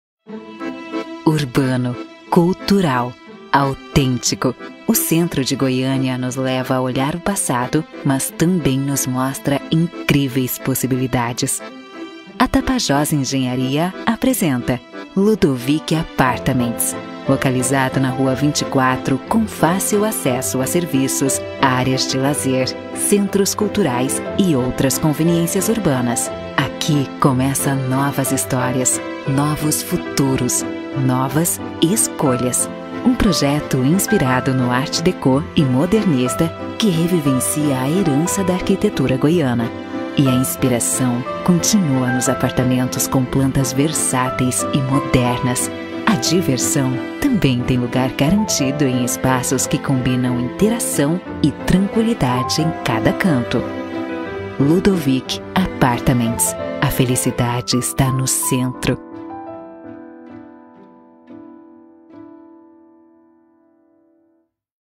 Lançamento Ludovic Apartments - Tapajós - Setor Central, Goiânia, GO
Invista em apartamentos na planta: lançamento no Setor Central em Goiânia, GO. Apartamentos de 2 a 3 quartos e área de 58 a 97 m².

MySide: o melhor amigo do comprador de imóveis
CRECI-SC 7220
CRECI-GO 30880
CRECI-PR 9492